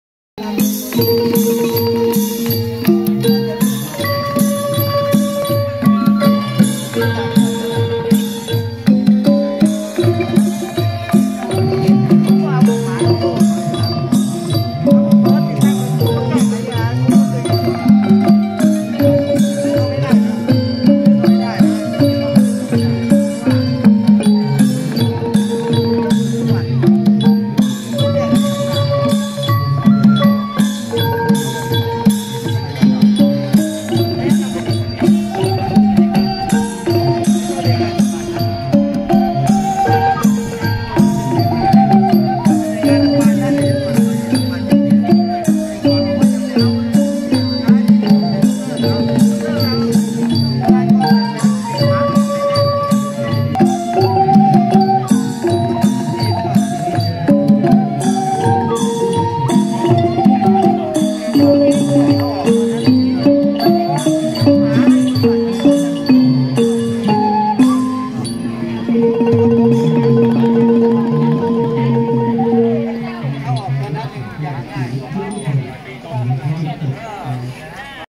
Thai music band Sisaket wittayalai band Thai song วงสิริมหาชุมพล โรงเรียนศรีสะเกษวิทยาลัย จ.ศรีสะเกษ
Thai music band Sisaket wittayalai band วงสิริมหาชุมพล โรงเรียนศรีสะเกษวิทยาลัย จ.ศรีสะเกษ บรรเลงเพลงไทย บรรเลงดนตรีไทยวงสิริมหาชุมพล นุ่งผ้าไทยใส่บาตร เกาะกลางน้ำ จ.ศรีสะเกษ